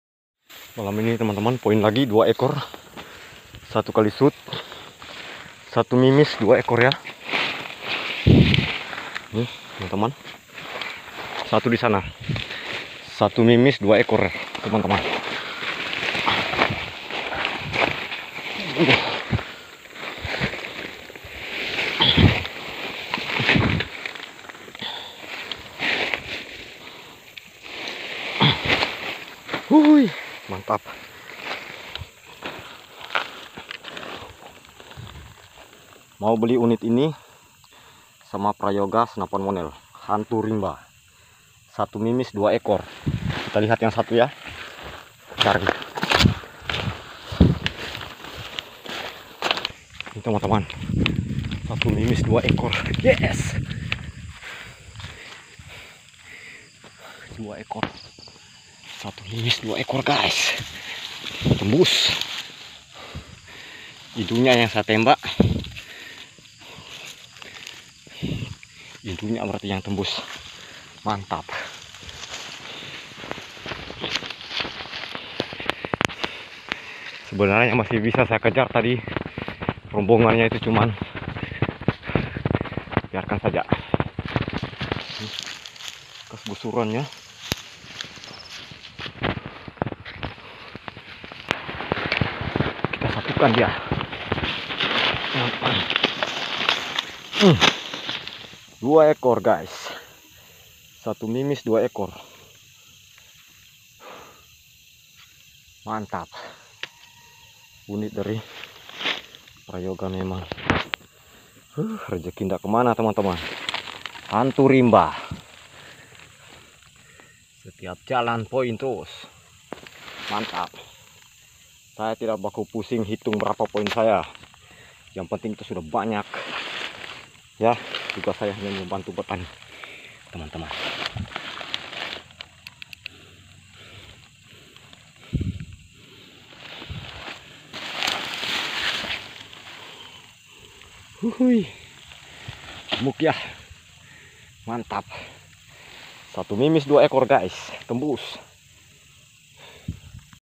rejeki lagi satu mimis dua ekor babi hutan satu kali shot@BraveHunterSangihe
berburu babi hutan babinya di tembak satu tembus di babi lainnya jadi rejeki malam ini sangat memuaskan hasil satu mimis dua ekor babi#babihutan